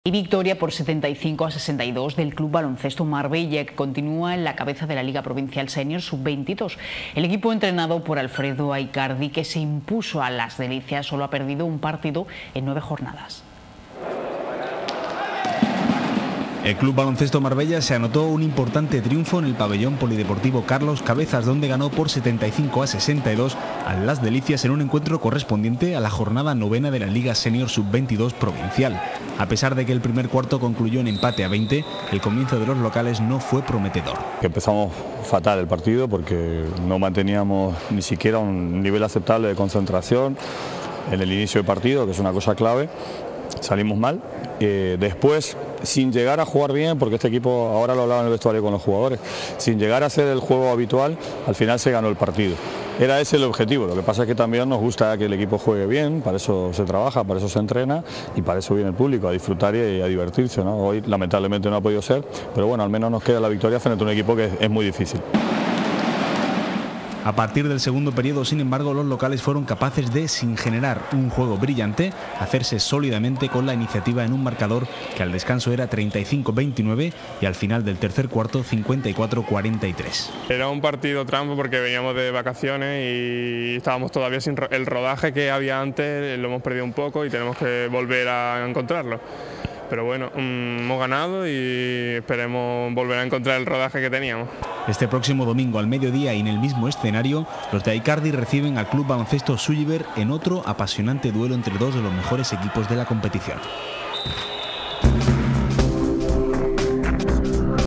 Noticias RTV Marbella - Club Baloncesto Marbella
Noticias sobre el C.B. Marbella en el informativo de RTV Marbella del 12 de enero de 2015 con la victoria del senior sub22 sobre Neumáticos Las Delicias de Málaga.